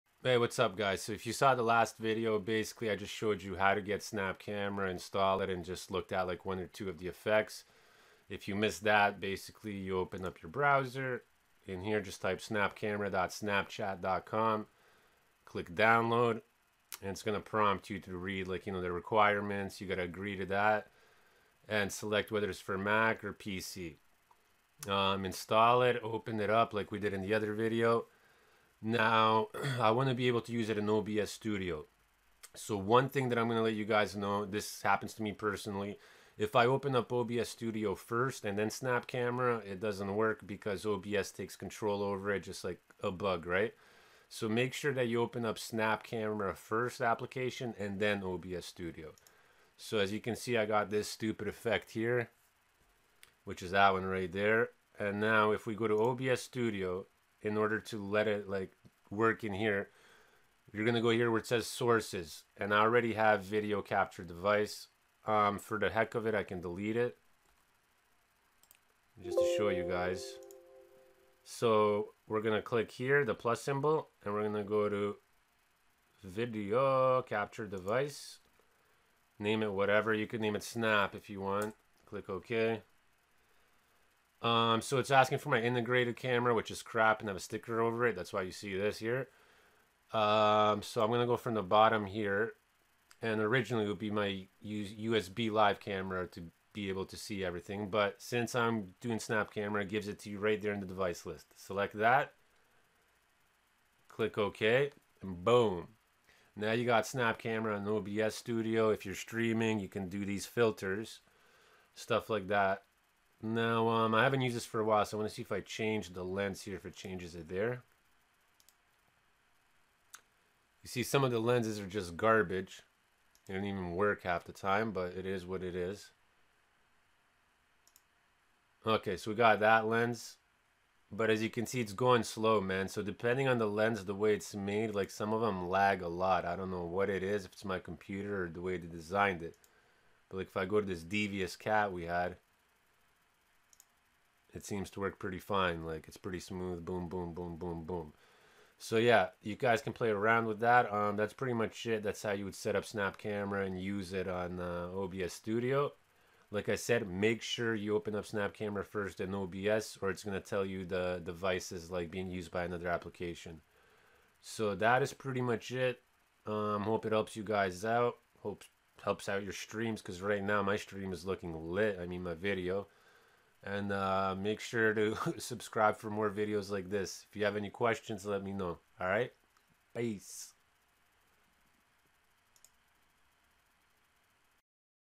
How to Use Snap Camera on OBS Studio
In this video I will be showing how to use snap camera in obs studio.